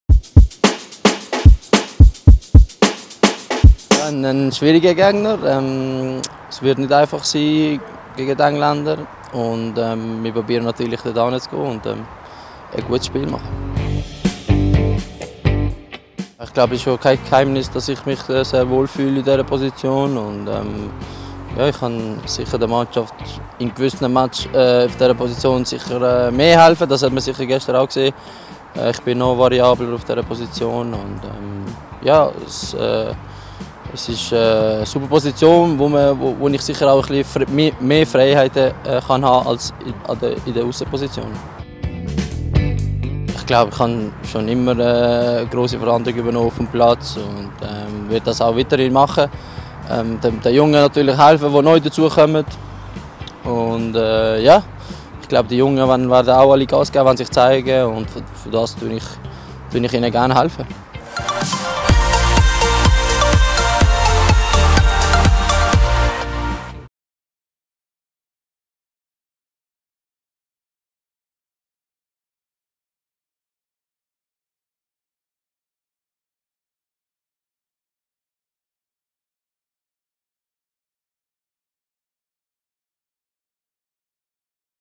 Exklusives Spieler-Video: Xherdan Shaqiri vor dem Testspiel Schweiz vs. England
Xherdan Shaqiri über den starken Gegner, seine neue 10er-Position als Spielmacher und seine Verantwortung im Nati-Kader.